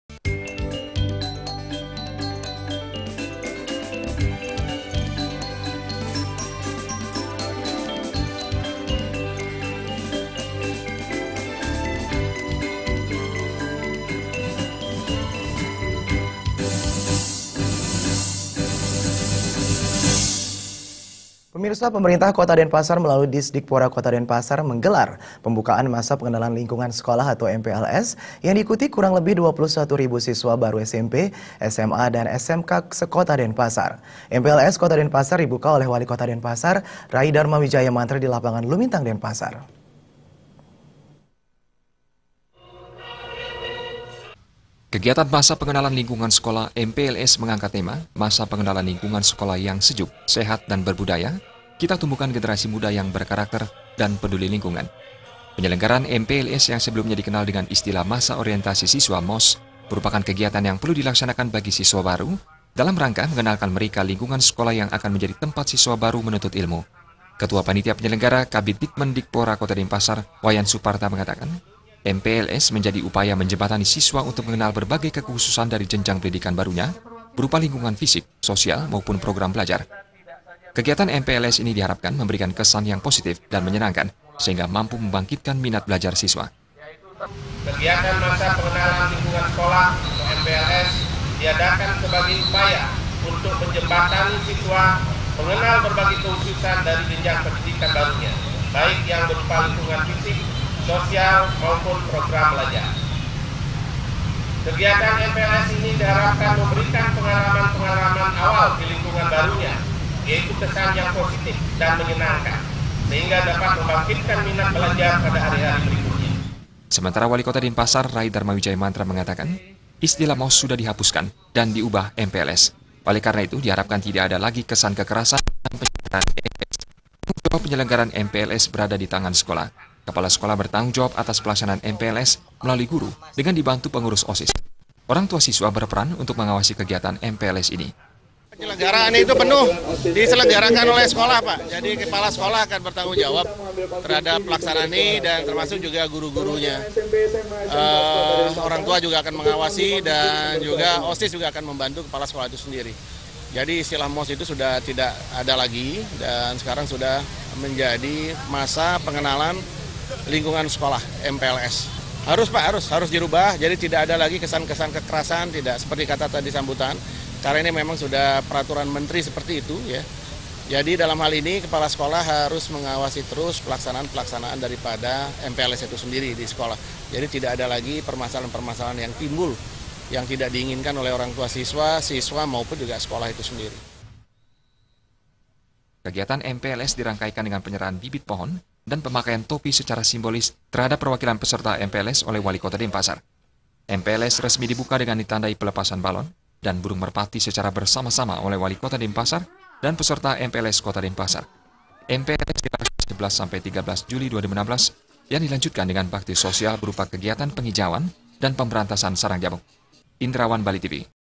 PEMBUKAAN MPLS KOTA DENPASAR
PEMERINTAH KOTA DENPASAR MELALUI DISDIKPORA KOTA DENPASAR, MENGGELAR PEMBUKAAN MASA PENGENALAN LINGKUNGAN SEKOLAH, MPLS, YANG DIIKUTI KURANG LEBIH 21.000 SISWA BARU SMP, SMA DAN SMK SEKOTA DENPASAR. MPLS KOTA DENPASAR DIBUKA OLEH WALIKOTA DENPASAR RAI DHARMAWIJAYA MANTRA DI LAPANGAN LUMINTANG DENPASAR.